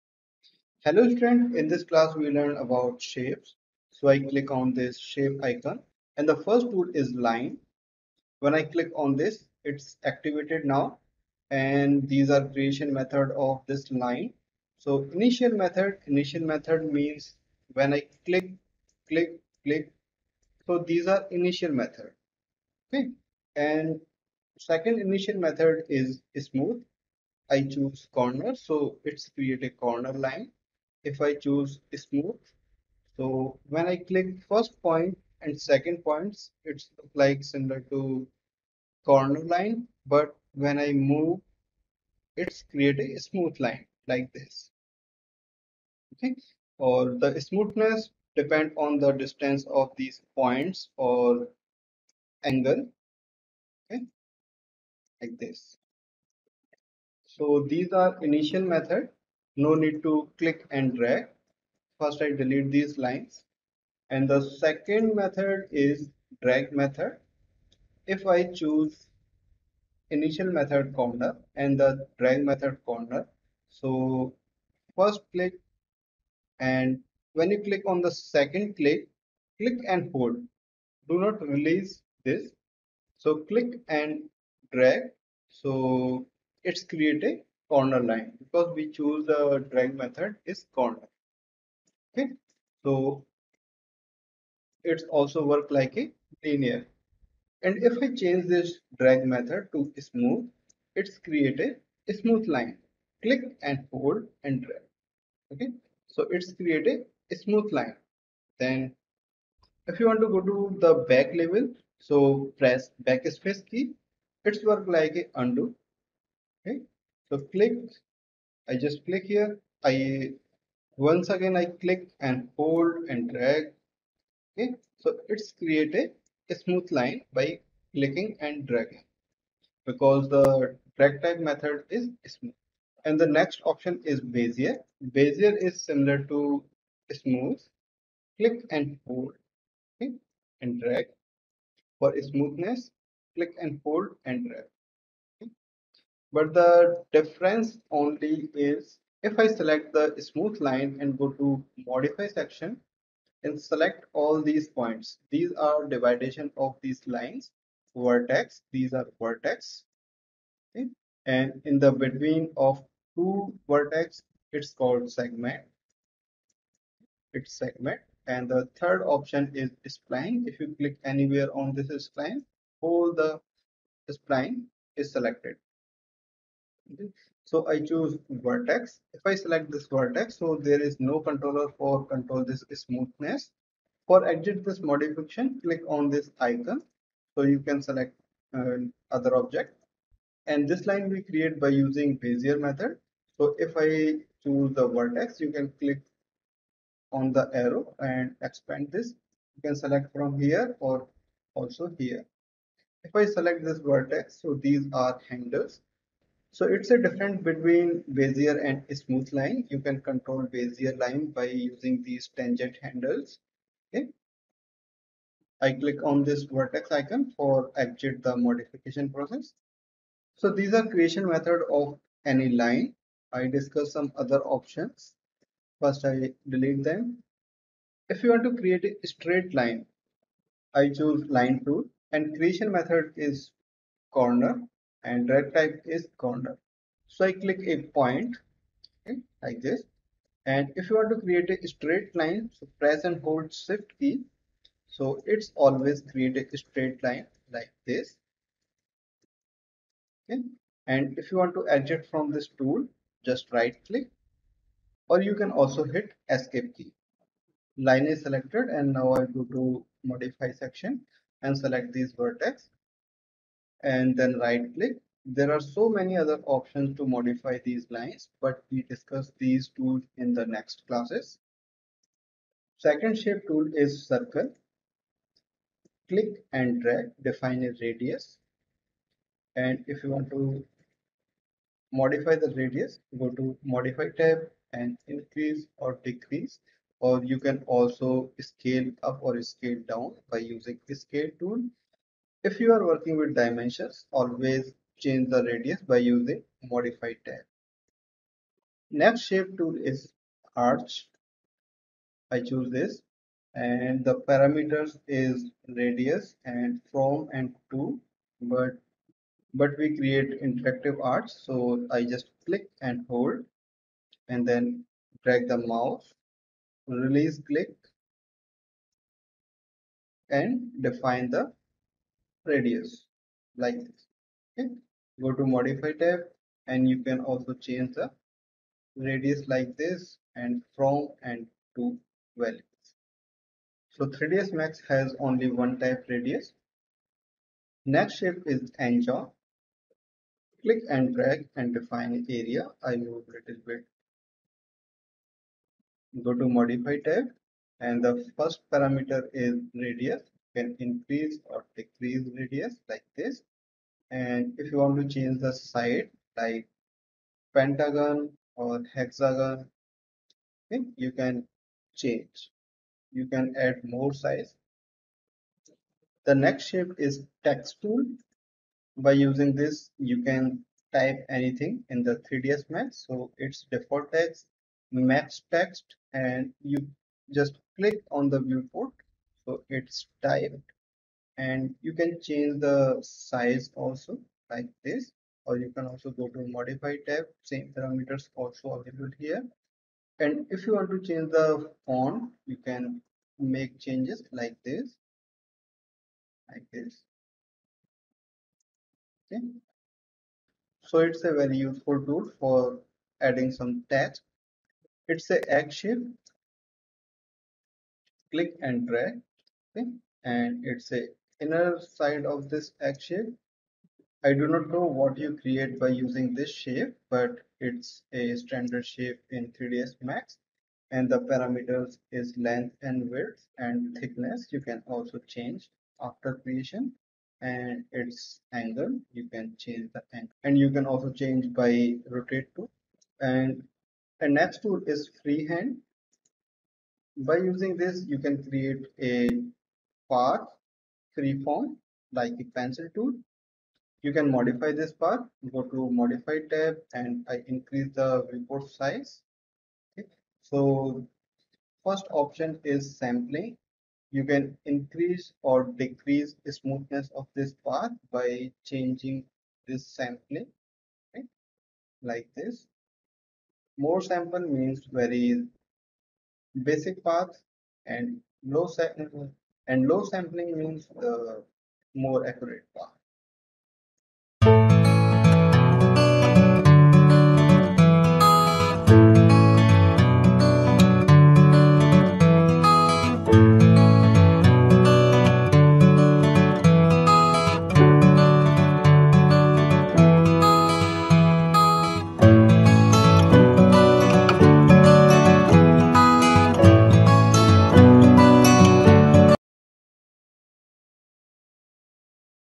Shapes in 3ds Max Pt. 01 | 3ds Max Training for Beginners
If you find all the content I create helpful, please consider supporting me!

You'll get early access to all videos before they go live on YouTube!

In this class we cover Shape with all parameters. Line Tool, Circle Tool,
Arc Tool, NGon Tool, Text Tool, Egg Tool,
Freehand Tool, Rectangle Tool, Ellipse Tool, Donut Tool, Star Tool, Helix Tool, Section Tool